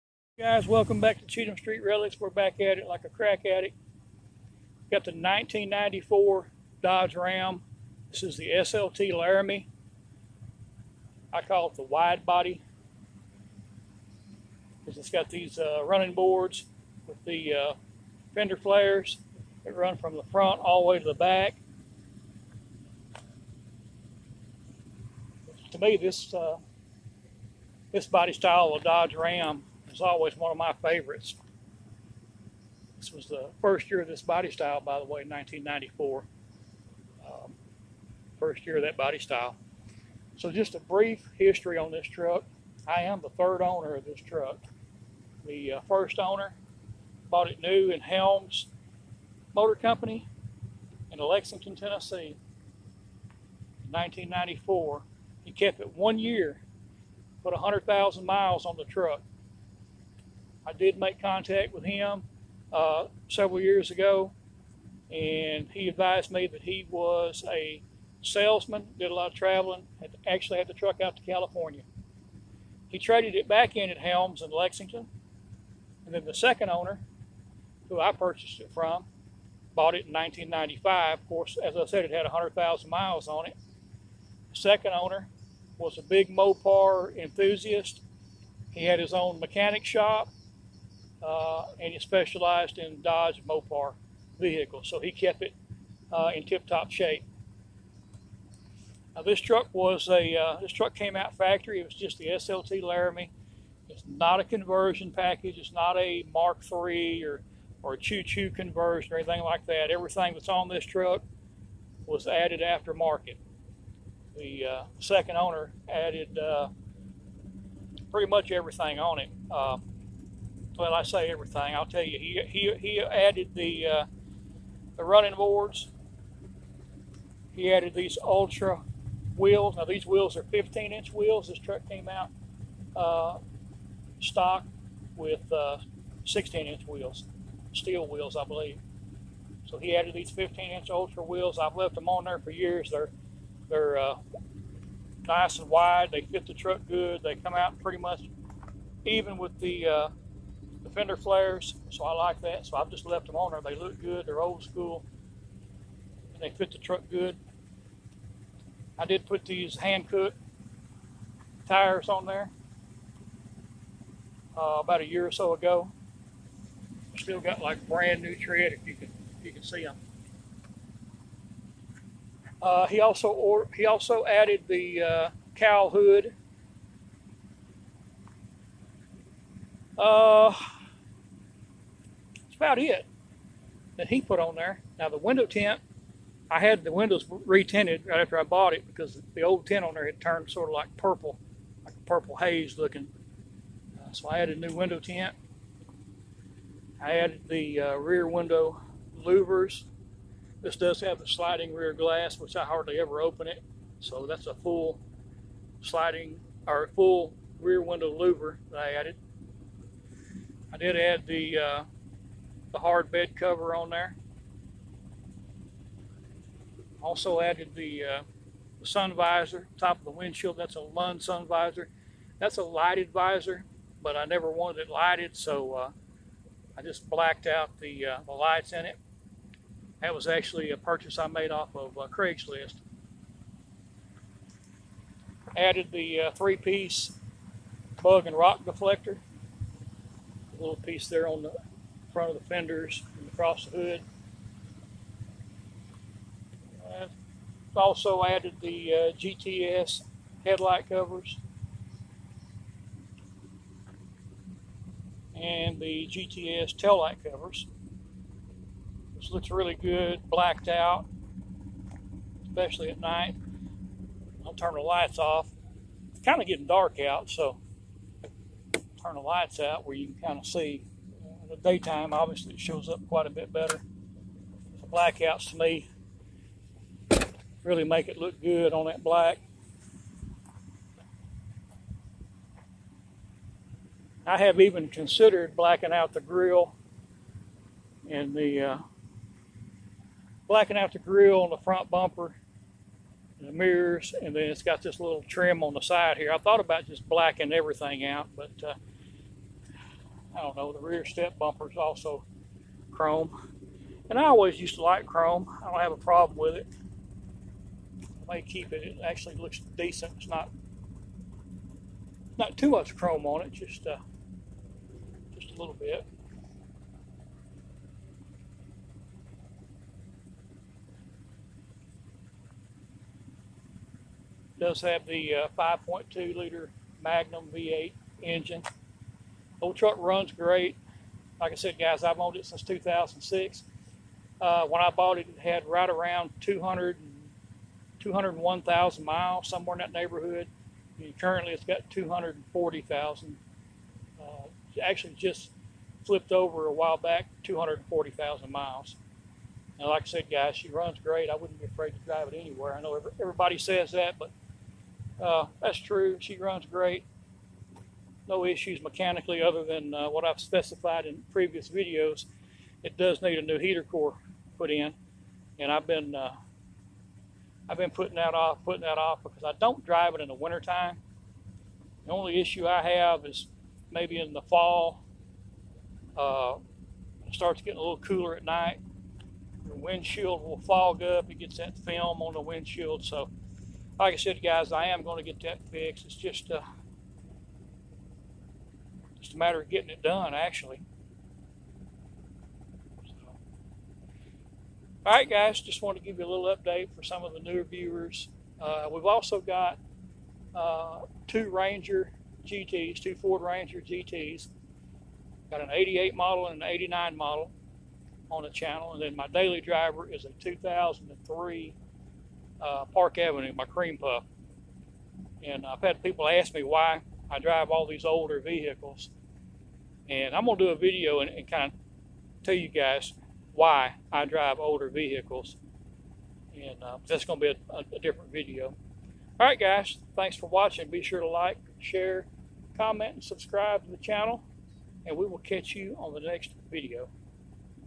1994 Dodge Ram 1500 SLT Walk Around (Wide-Body)
Early evening walk around of our '94 Dodge Ram 1500 SLT. 💥